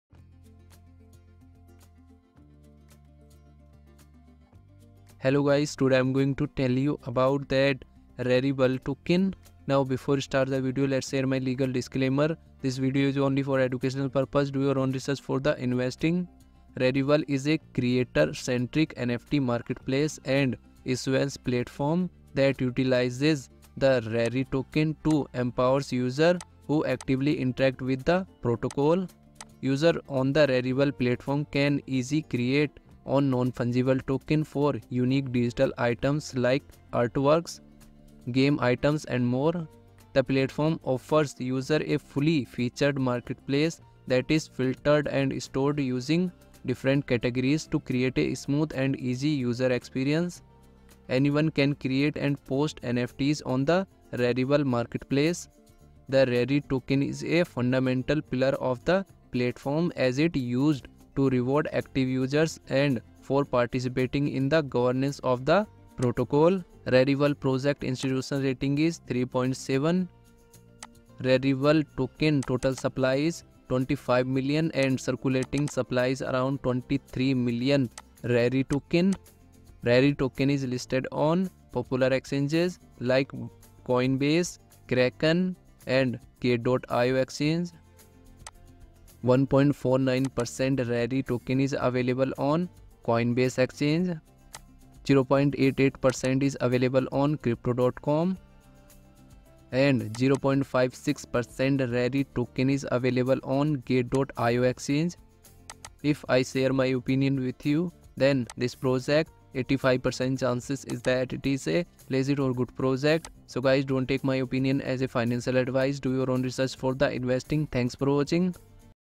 What is Rarible (RARI) Coin | Review About RARI Token
In this video, I will share a Review of the Rarible (RARI) token. I have collected all the data from online sources please don't take this video as financial advice. I am not a financial advisor.